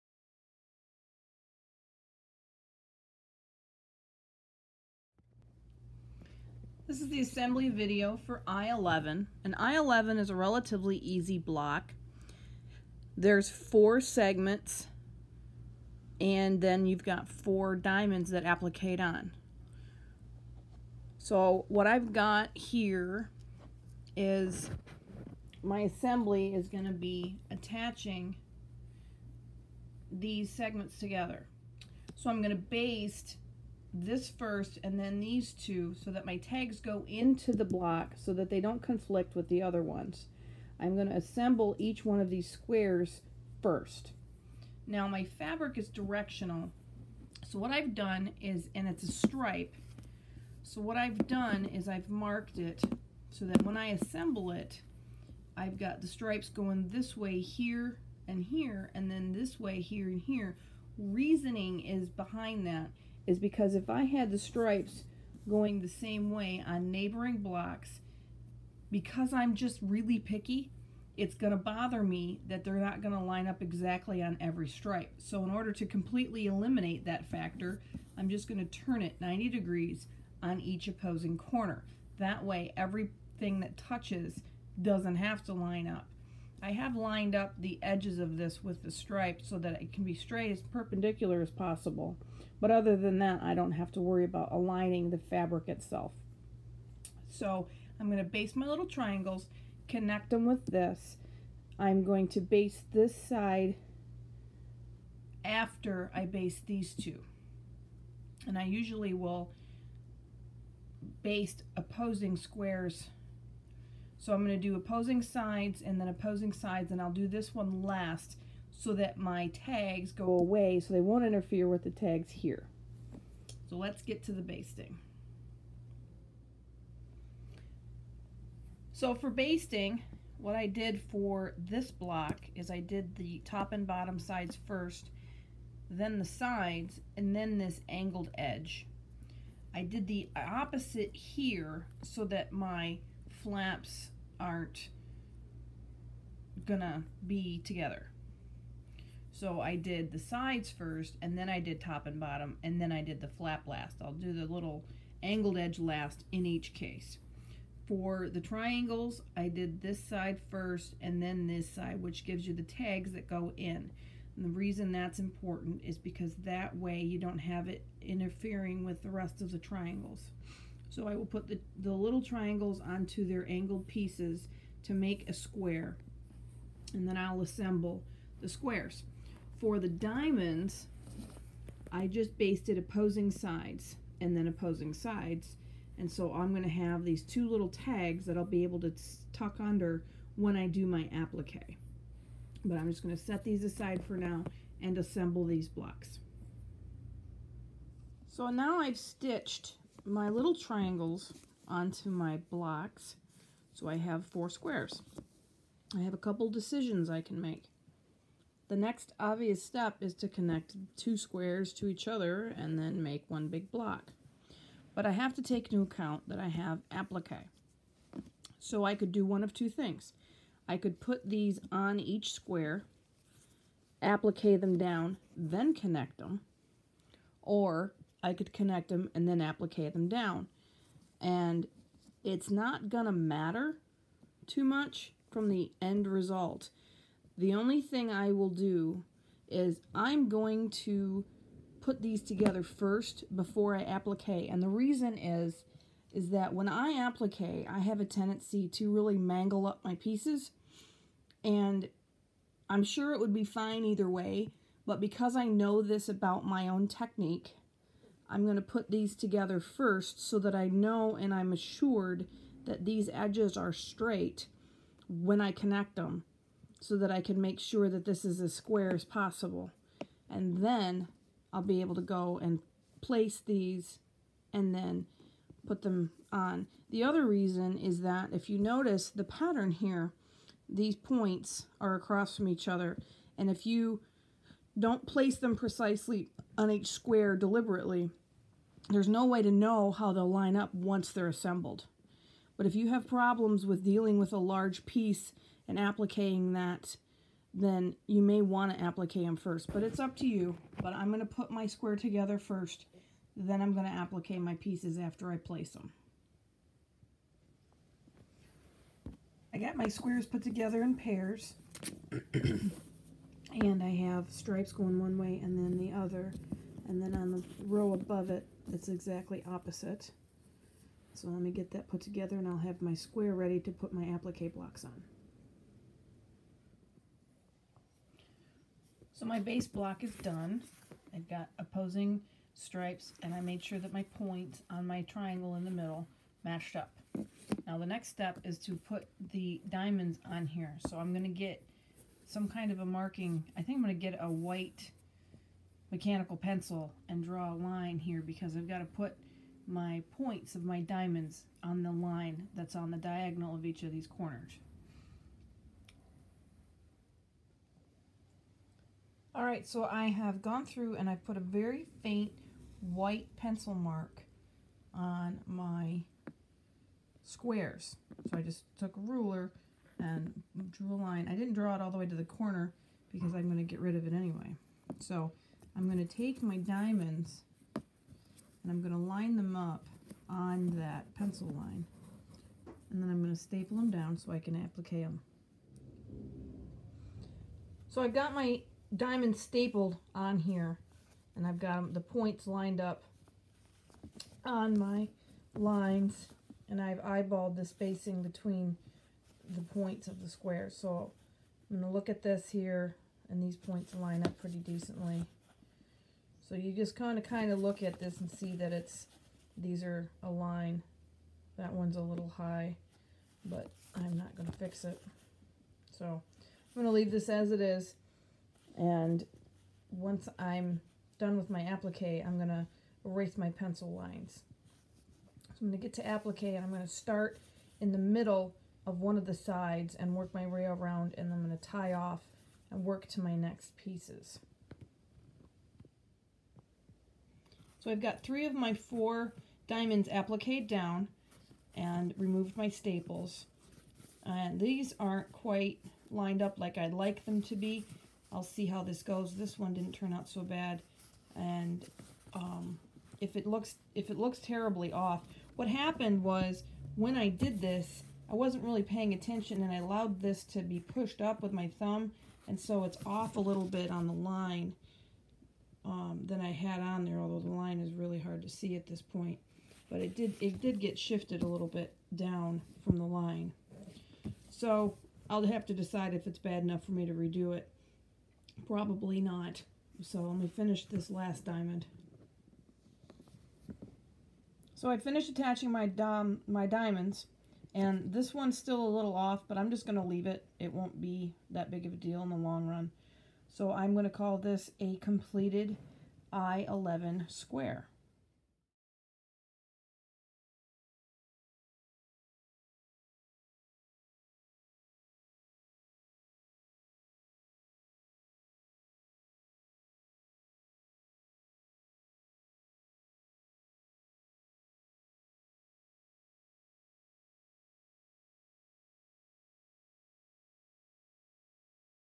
I11 Block Assembly - Dear Jane Goes EPP
Step by step instructions for assembling the I11 block of the Dear Jane quilt using the EPP kit available from Paper Pieces.